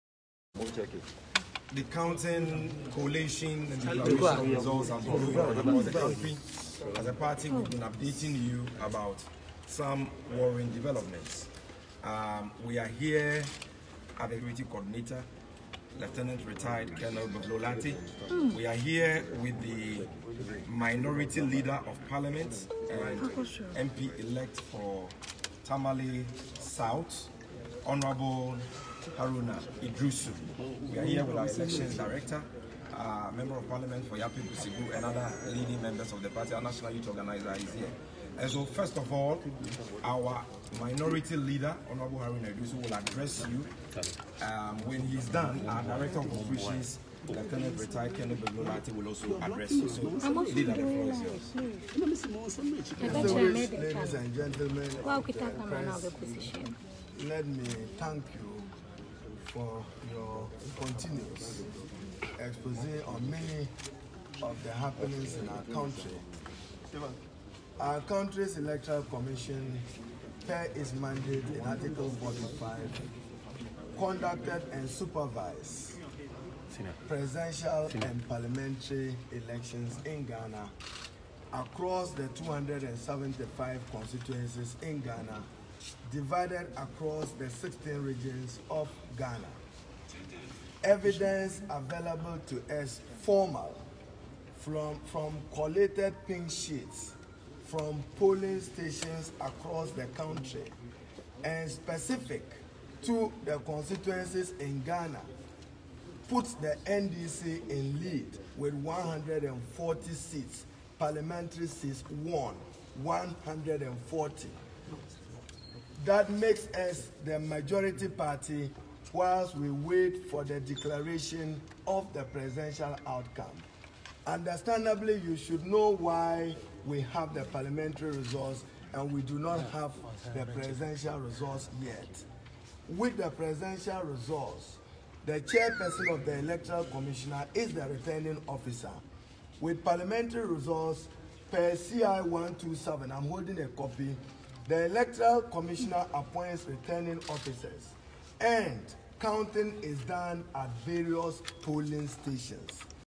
Live- Sammy Gyamfi And Minority Leader of Parliament Haruna Iddrisu Says NDC Leads In Parliament
As EC is set to declare the 2020 elections results. Communication head of NDC Sammy Gyamfi and Minority leader in parliament Haruna Iddrisu has affirmed that, they (NDC) continue to lead majority in parliament.